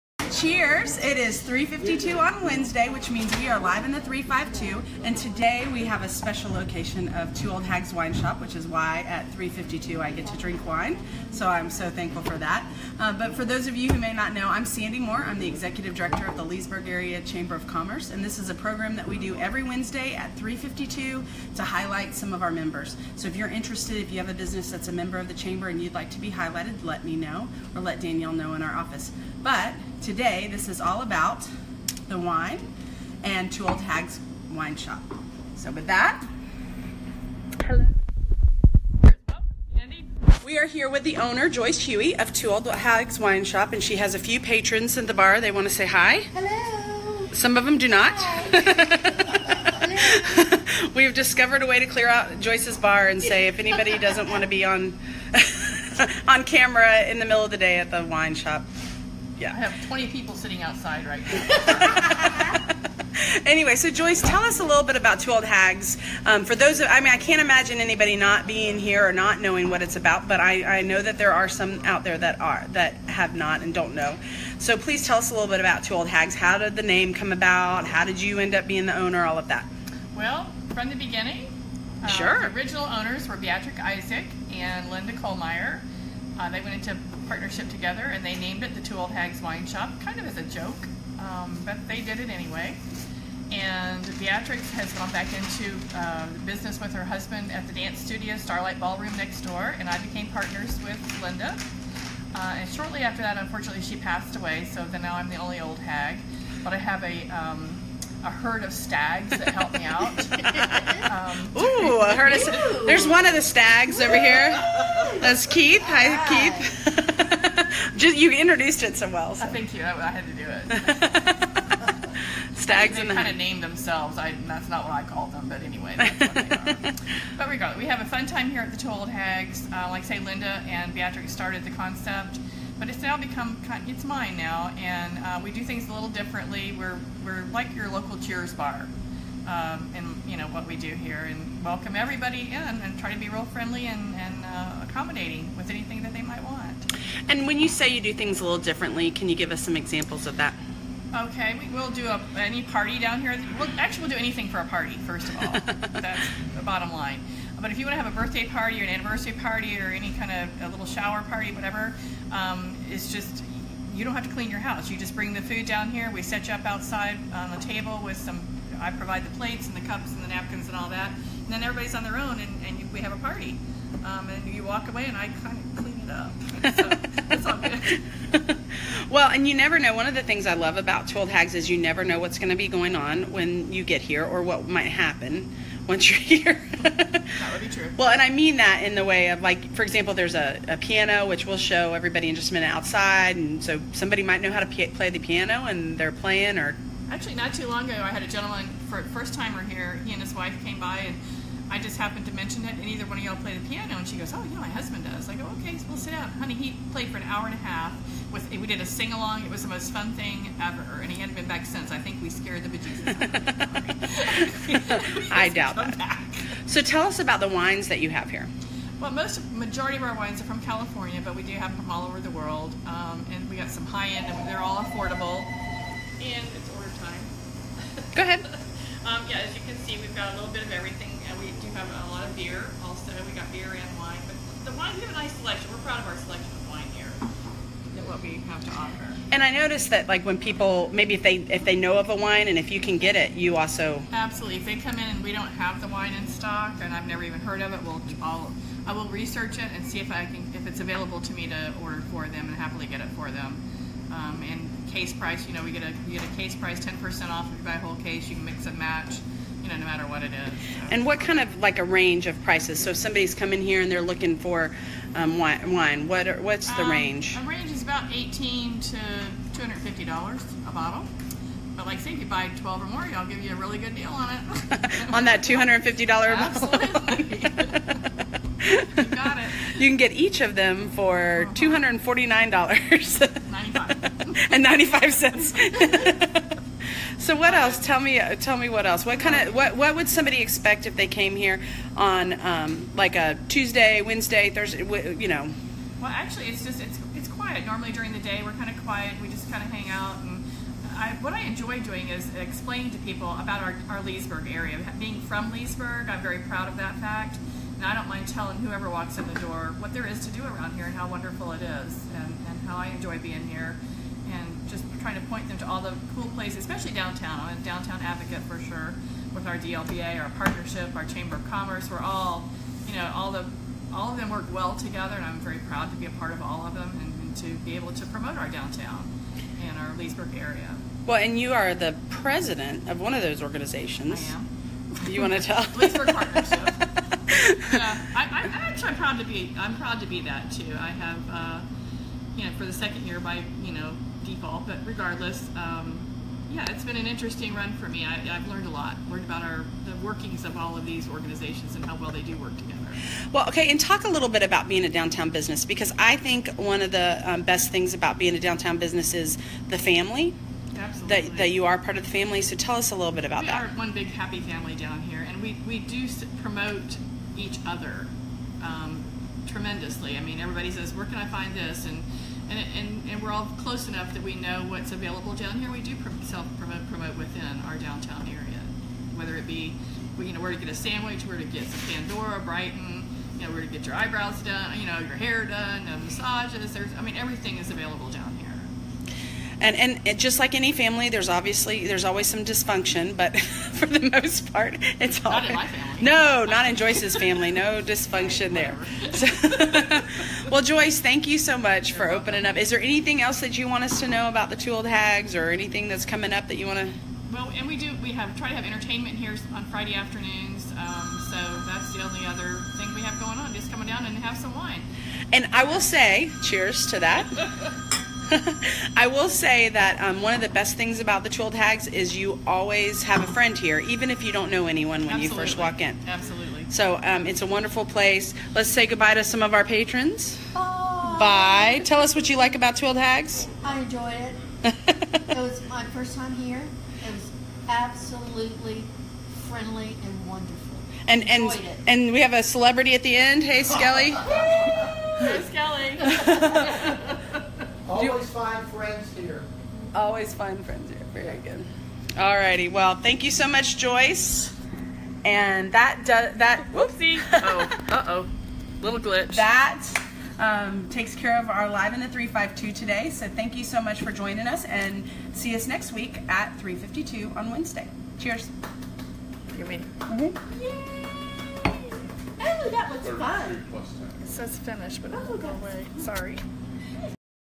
Live in the (352) with Two Old Hags
Leesburg Florida's very own wine bar. Whether you are in need of a glass of wine after work or a bottle, Two Old Hags is where you want to go. A locally owned business in the heart of historic downtown Leesburg.